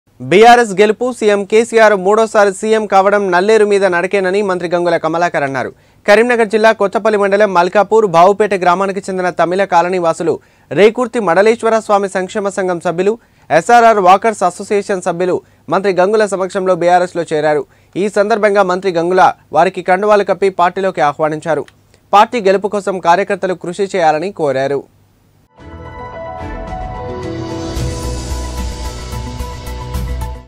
సీఎం కేసీఆర్ మూడోసారి సీఎం కావడం నల్లేరు మీద నడకే || మంత్రి గంగుల కమలాకర్
బీఆర్ఎస్ గెలుపు.. సీఎం కేసీఆర్ మూడోసారి సీఎం కావడం నల్లేరు మీద నడకేనని మంత్రి గంగుల కమలాకర్ అన్నారు.// కరీంనగర్ జిల్లా కొత్తపల్లి మండలం మల్కాపూర్, బావుపేట గ్రామానికి చెందిన తమిళ కాలనీవాసులు, రేకుర్తి మడలేశ్వరస్వామి సంక్షేమ సంఘం సభ్యులు, ఎస్ఆర్ఆర్ వాకర్స్ అసోసియేషన్ సభ్యులు మంత్రి గంగుల సమక్షంలో బీఆర్ఎస్ లో చేరారు.// ఈ సందర్భంగా మంత్రి గంగుల వారికి కండువాలు కప్పి పార్టీలోకి ఆహ్వానించారు.//  పార్టీ గెలుపు కోసం కార్యకర్తలు కృషిచేయాలని కోరారు ||@S6News